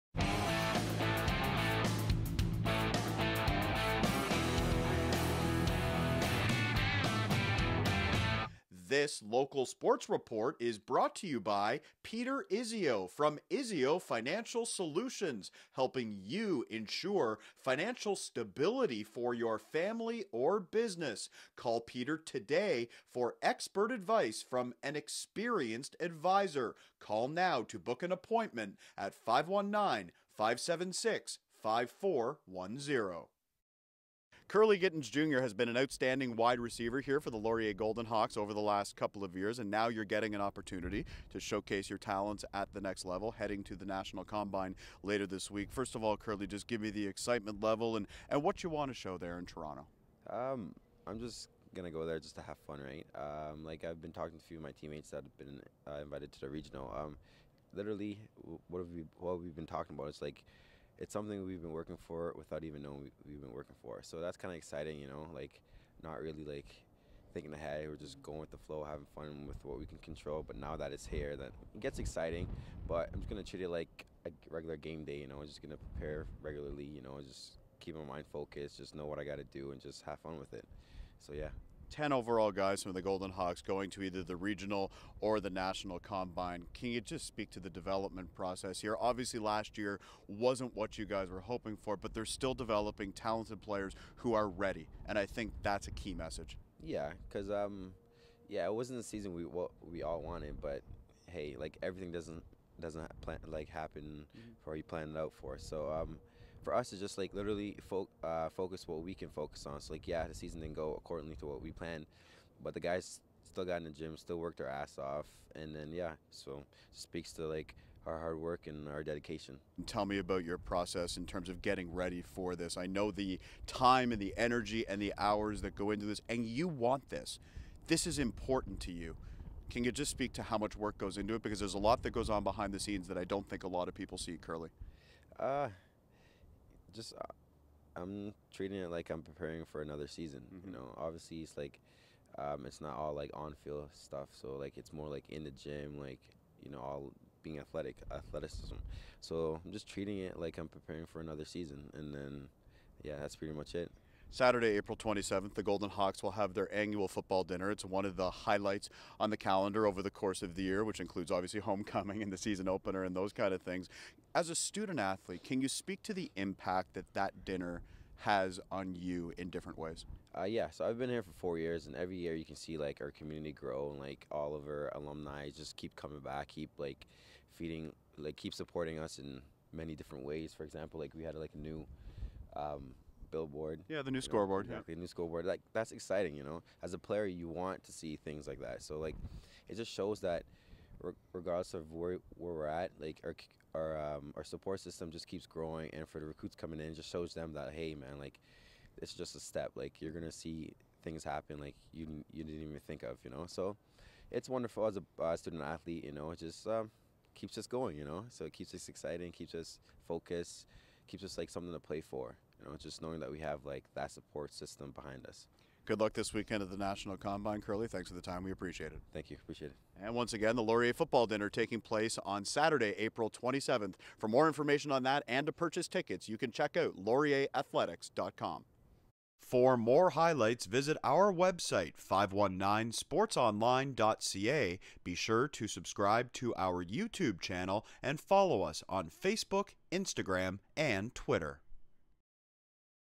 OUA Football - Kurleigh Gittens Jr. heading to CFL combine this weekend
Gittens Jr. is one of 10 players from the Laurier Golden Hawks who will take part in the CFL's regional or national combine this week.